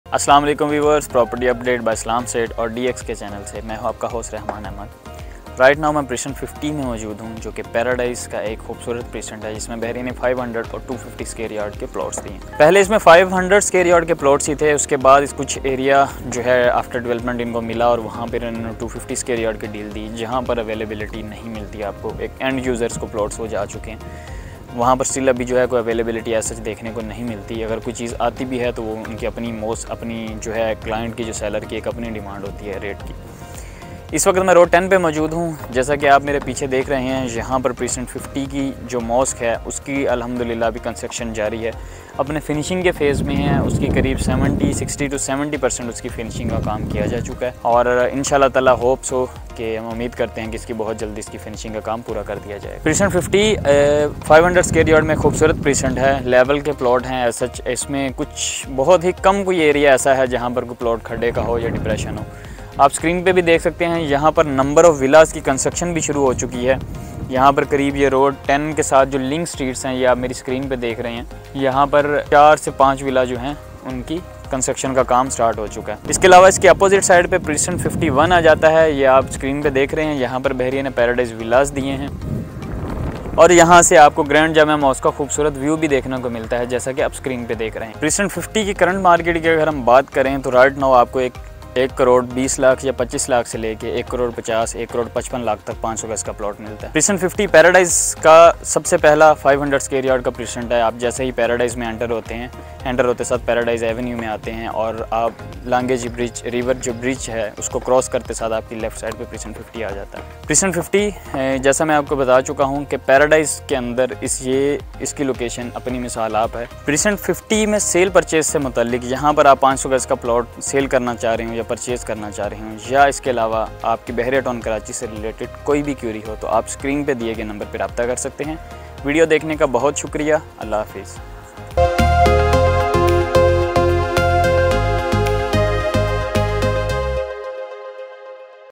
Precinct 50 Bahria Town Karachi | Bahria Paradise | 250-500 Sq yard | Latest Updates and Status
Bahria Paradise's Precinct 50 has 500 square yard plots. Work has begun on the mosque. The land has been completely leveled. There are around four or five villas being constructed here. The current price range of precinct 50 is between 1 crore 20 lac to 1 crore 50 lac. Get in touch with us for more information.